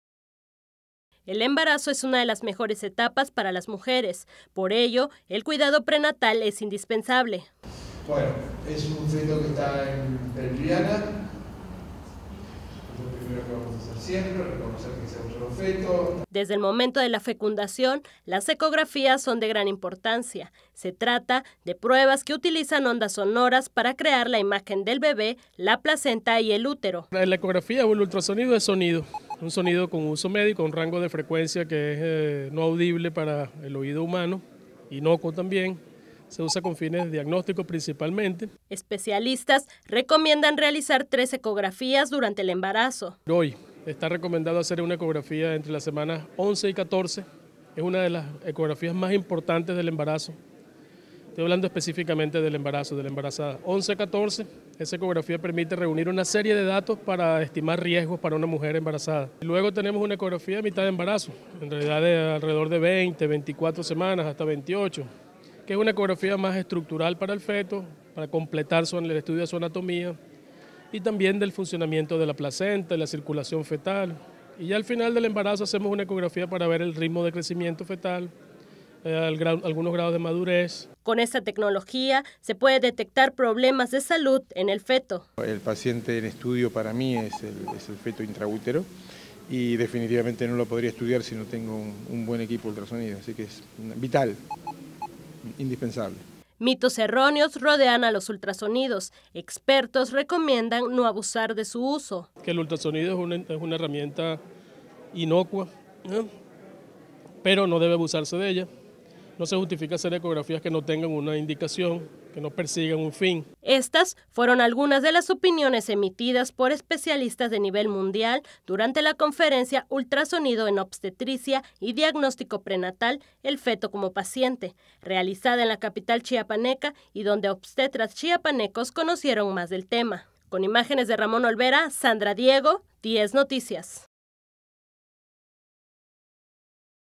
Realizan conferencia "Ultrasonido en Obstetricia y diagnóstico prenatal: El feto como paciente"
Desde el momento de la fecundación, las ecografías son de gran importancia, se trata de pruebas que utilizan ondas sonoras para crear la imagen del bebé, la placenta y el útero.
Especialistas recomiendan realizar tres ecografías durante el embarazo.